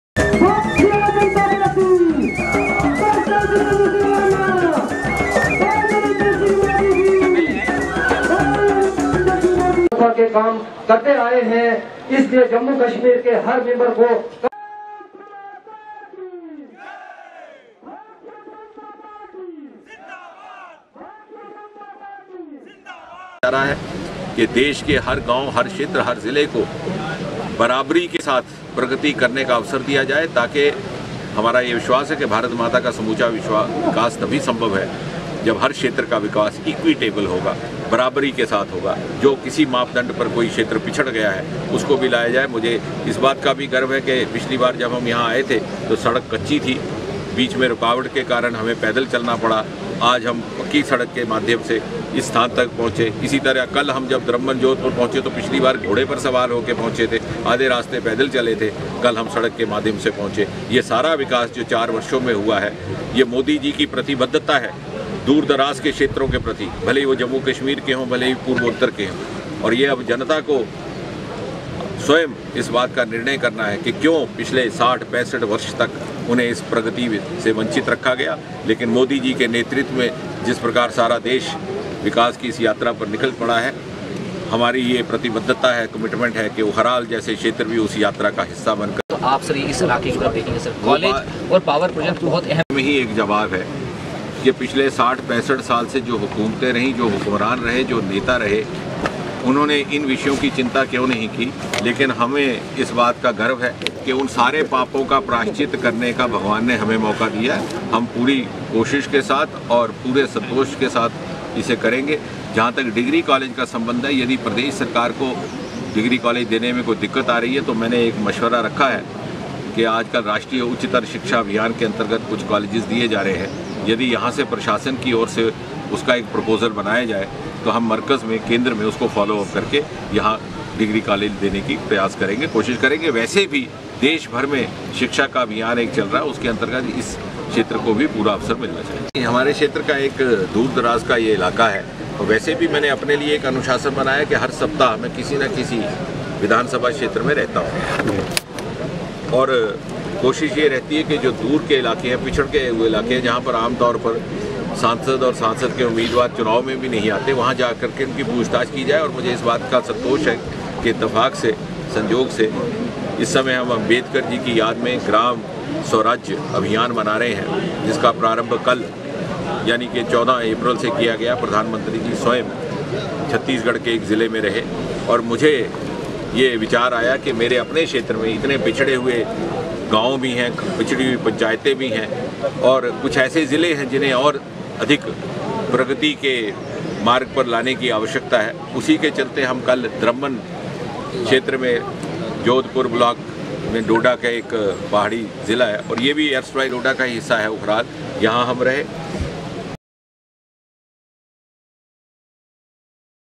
Jitendra Singh says people need to introspect for delayed development
| Excelsior News |